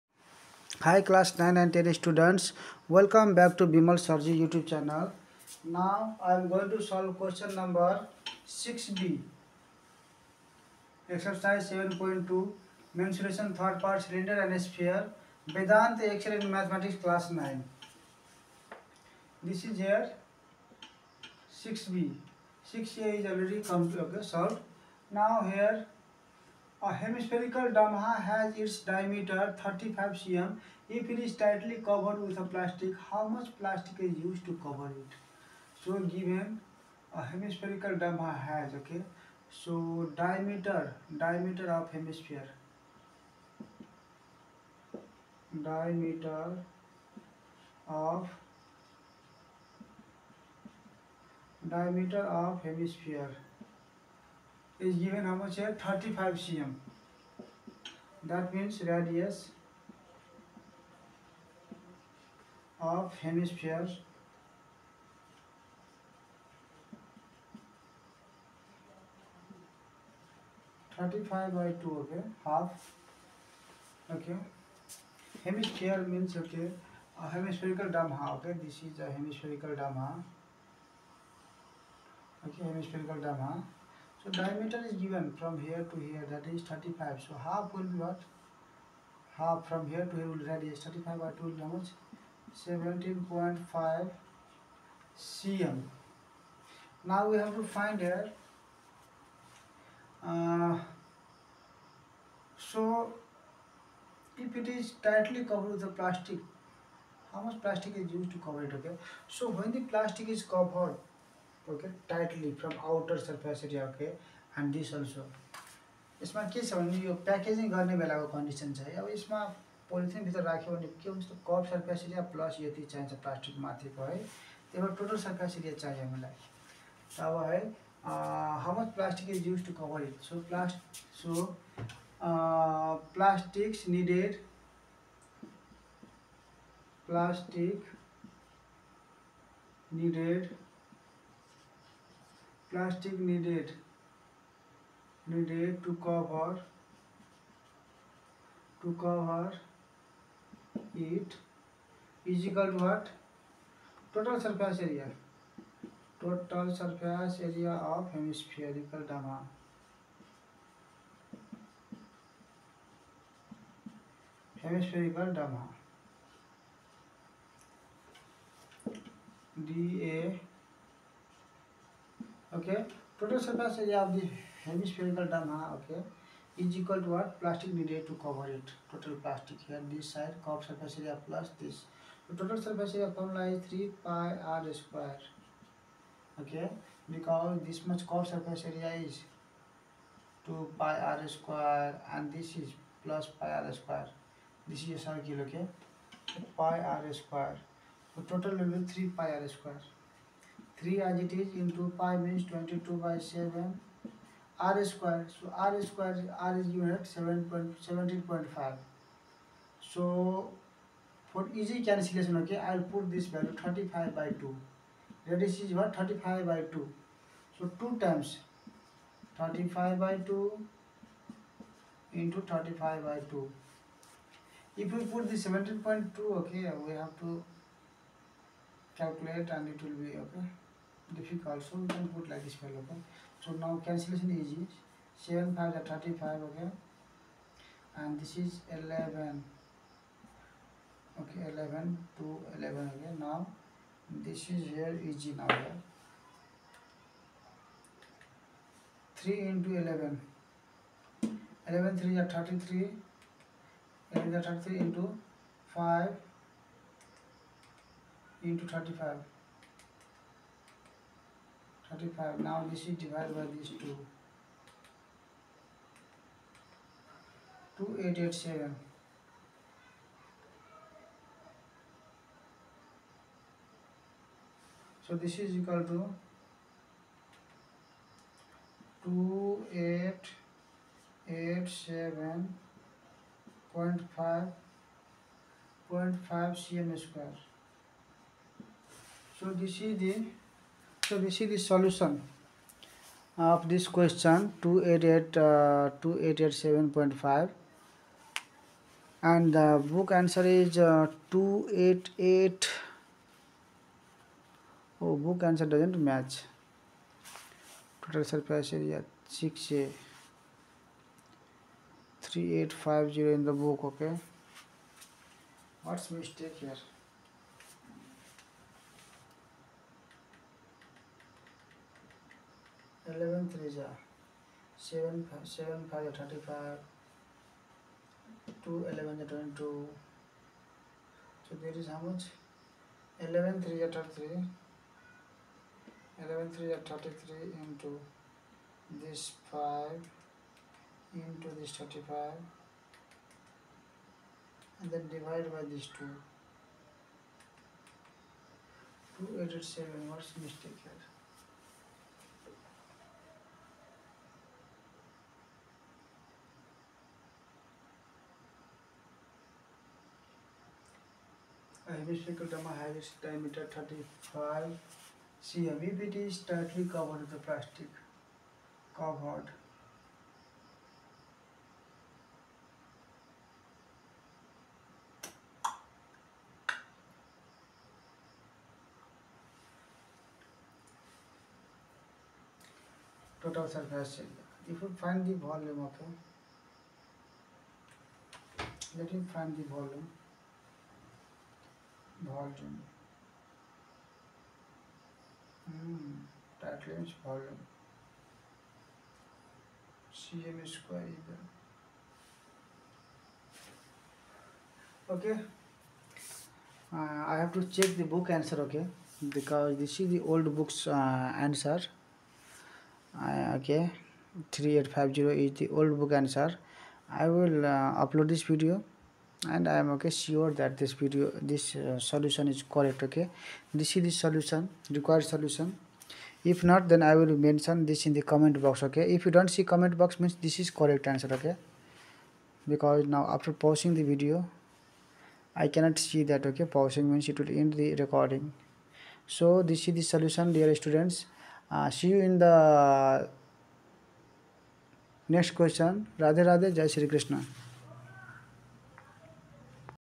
A hemispherical 'Damaha' has its diameter 35 cm. If it is tightly covered with a plastic, how much
A hemispherical 'Damaha' has its diameter 35 cm. If it is tightly covered with a plastic, how much plastic is used to cover it?

class 9 Ex- 7.2 Q6b) Mensuration (iii): Cylinder and Sphere Vedanta excel in mathematics class 9 and 10